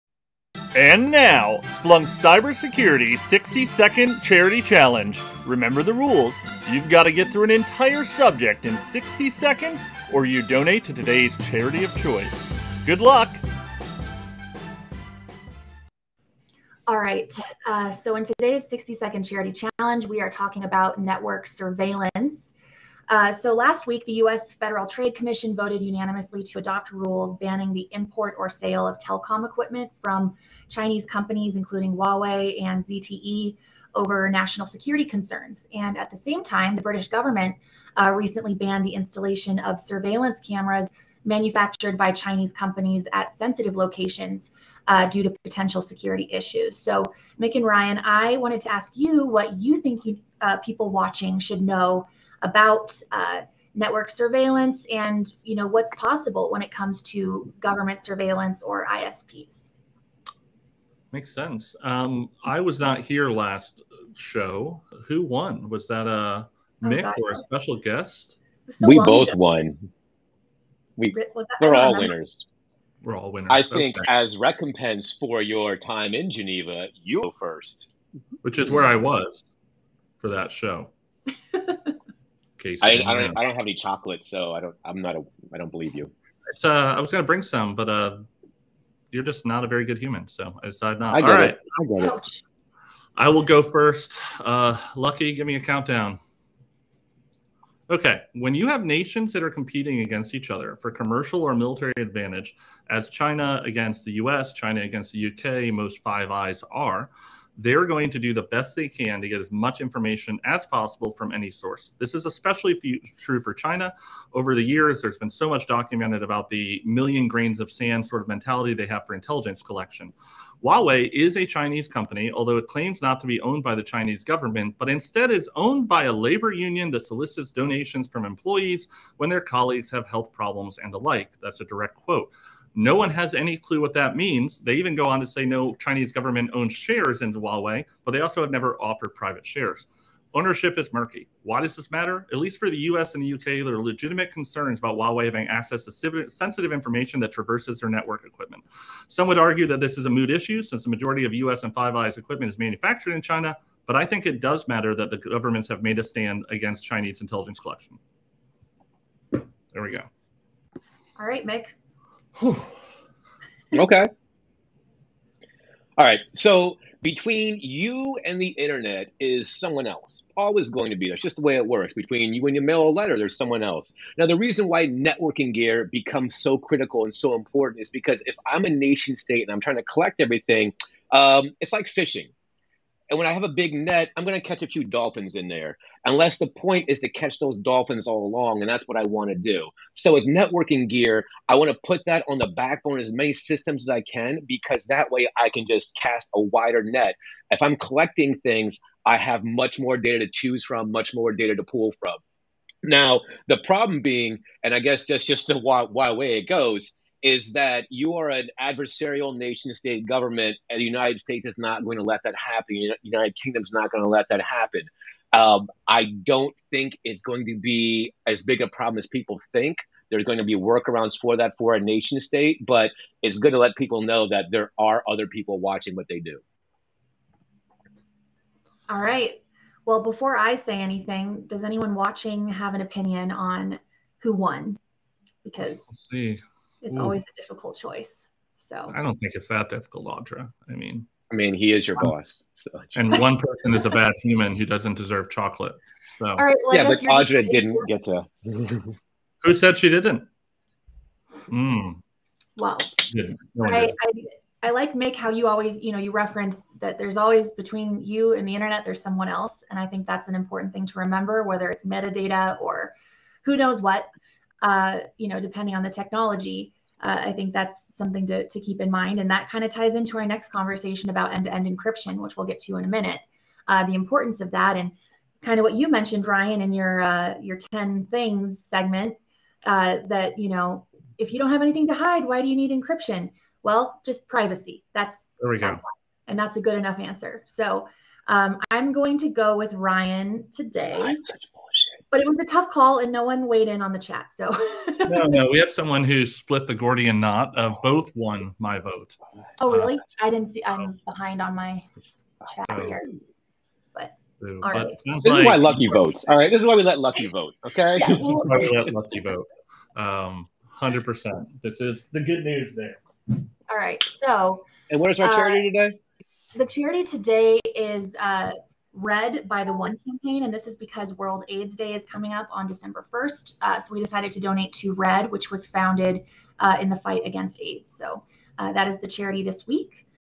SURGe 60 Second Charity Challenge: Network Surveillance
In this 60 second charity challenge benefitting RED by the ONE Campaign, Mick and Ryan shared their takes on network surveillance and national security concerns. You can watch the full Coffee Talk with SURGe episode in this playlist